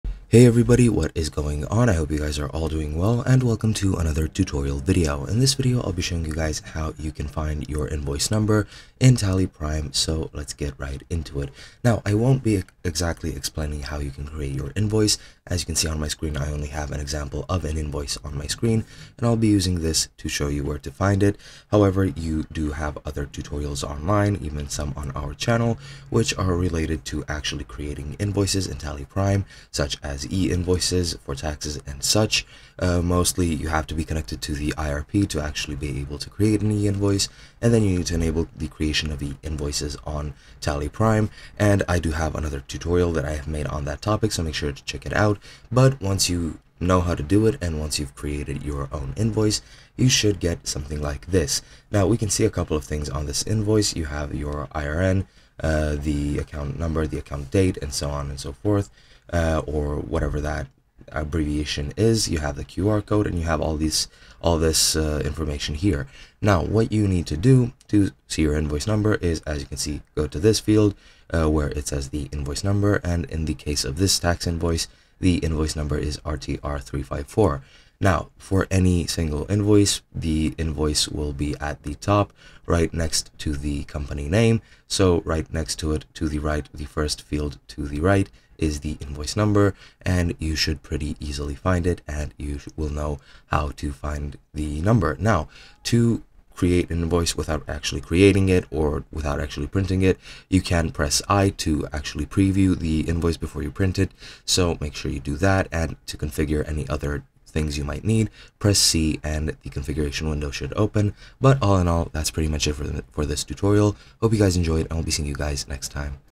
How To Find Invoice Number In Tally Prime Tutorial
Today we talk about find invoice number in tally prime,tally prime,learn tally prime,how to find invoice number in tally prime